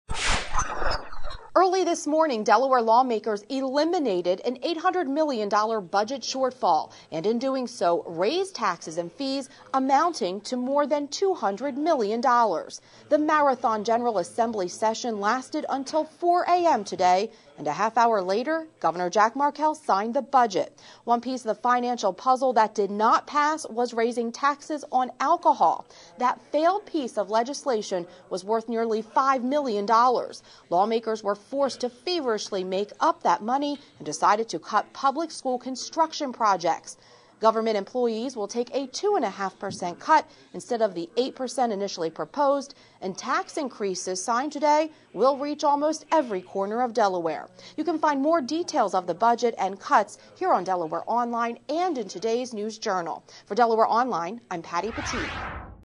State Budget passes in marathon session [Delaware Online Ne
Completing a marathon session that lasted until 4 a.m. today, the General Assembly passed a $3.09 billion budget, $206 million in tax and fee increases and eliminated an unprecedented $800 million revenue shortfall.